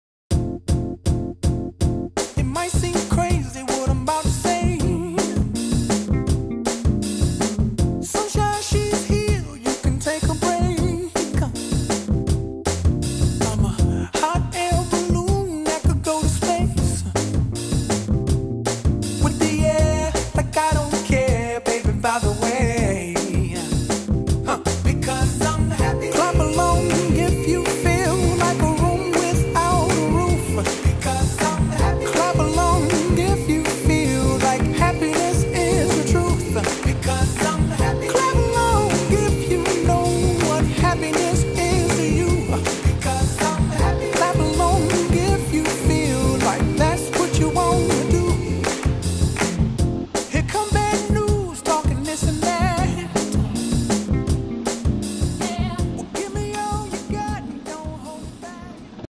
Mein 1. Schritt zum Führerschein
Auf der RehaCare erste mal Auto fahren.